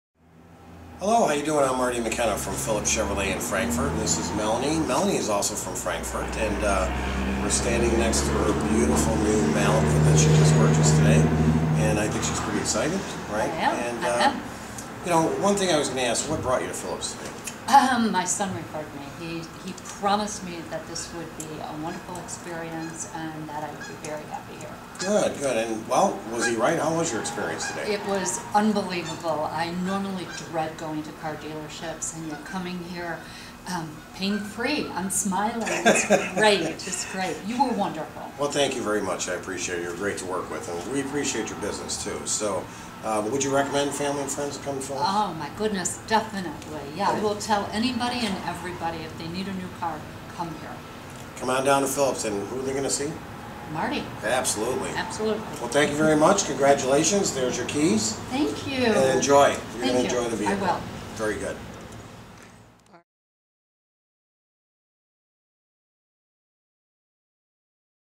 2013 Chevy Malibu - Customer Review Phillips Chevrolet - New Car Dealer Sales Chicago Dealership
- Here at Phillips Chevrolet in Frankfort, Illinois, we see a lot of new faces thanks to the good referrals from others!  Melanie, who was referred to us by her son, is taking home a brand new 2013 Chevy Malibu with the help of sales consultant, Marty McKenna.  Melanie says, "It was unbelievable.  I normally dread going to car dealerships, but coming here was pain free. Look, I'm smiling.  It was great." Stop in and see for yourself why we get recommended!  We're located at the corner of Routes 30 and 45 in Frankfort, or feel free to give us a call at .